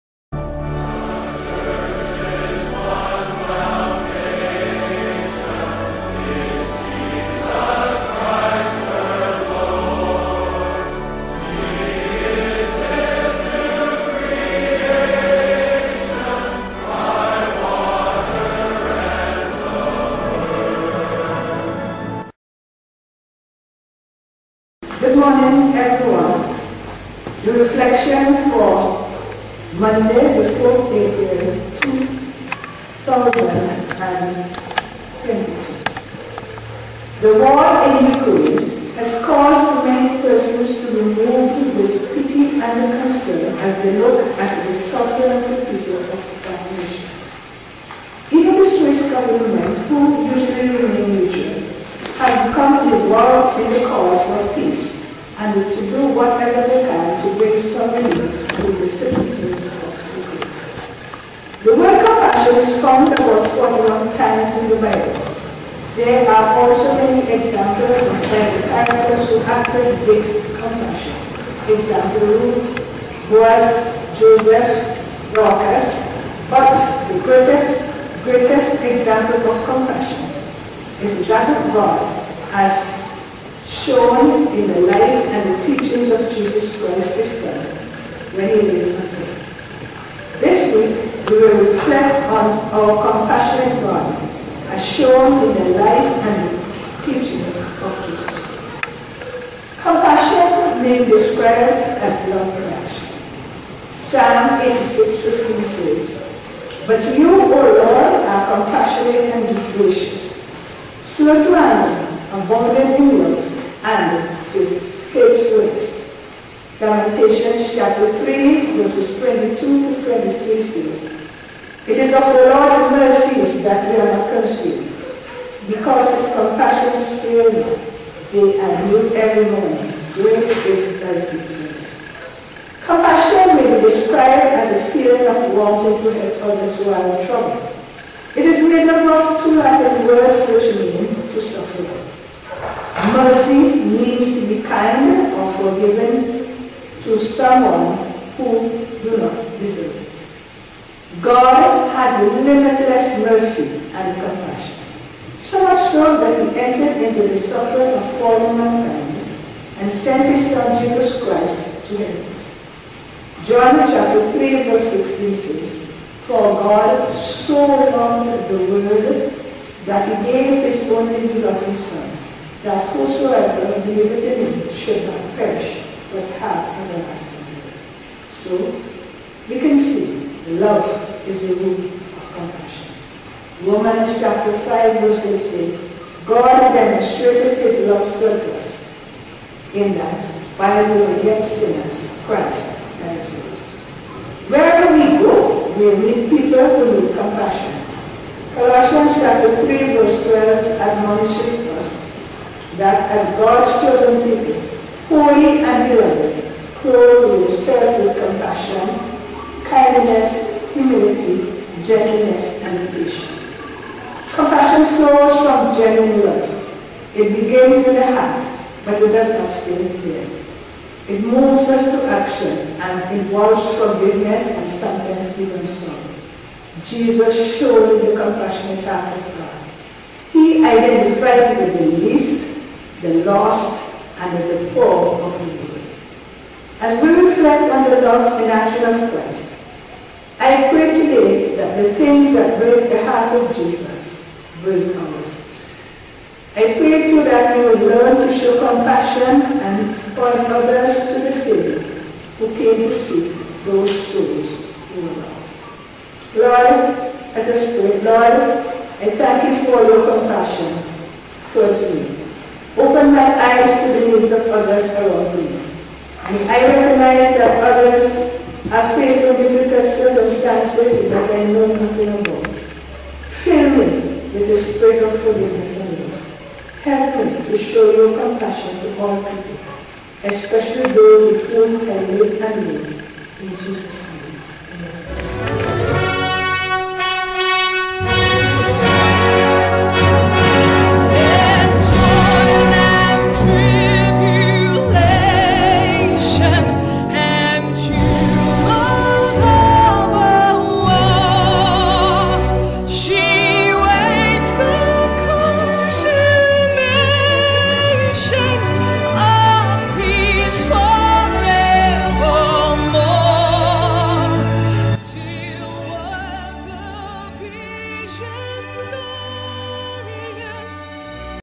Reflections For Lent Day 29 By Corps Sergeant Major (CSM) Paula Morris - Salvation Army
SVG Christian Council presentation